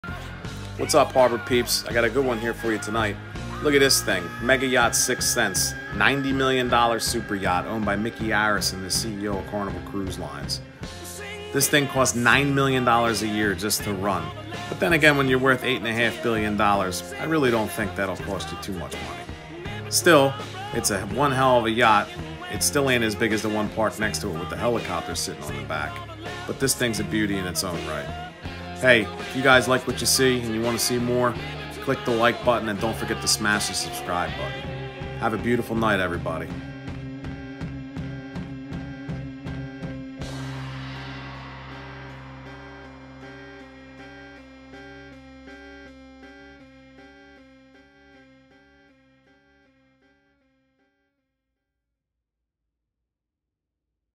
See the 90 million dollar Super Yacht “Sixth Sense”owned by billionaire Micky Arison!!!!
One of Evergreen’s new F class container ships. This thing is over 1000 feet long and carries over 12000 containers. Massive ship. Ever since they raised the Bayonne Bridge these behemoths can get in and out of the Port of Newark. Today she is heading out to sea.